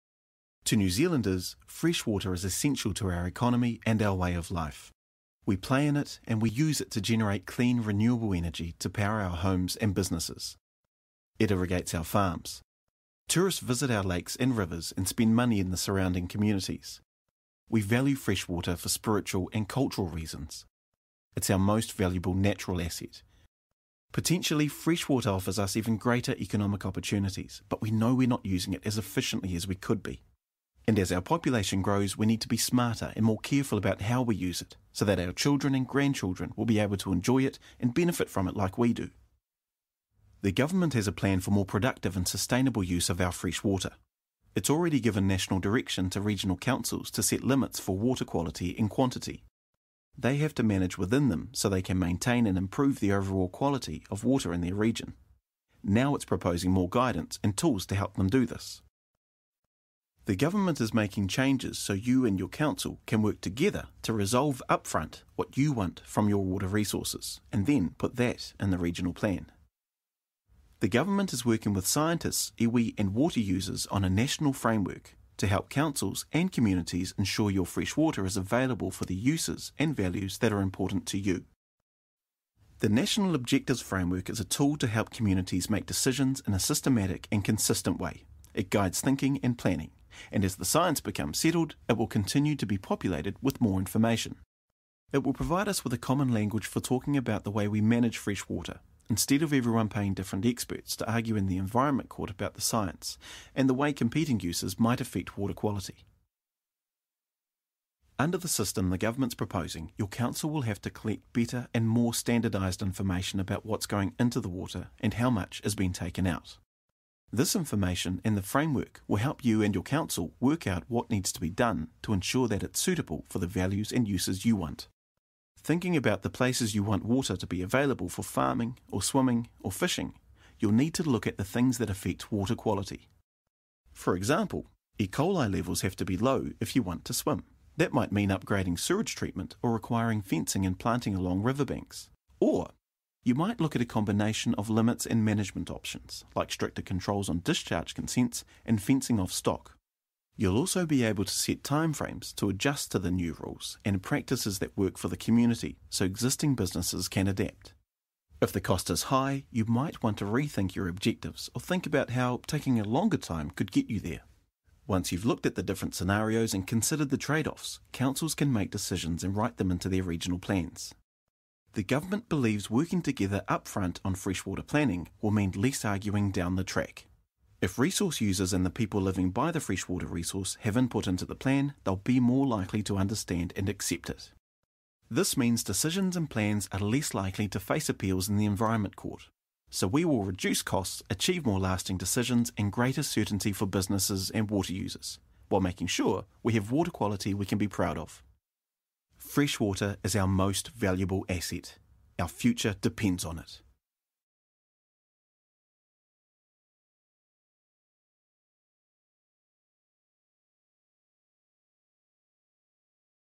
MfE Water Quality HD
The New Zealand Government is proposing a new way of managing our freshwater so we get more productive and sustainable use of this precious resource.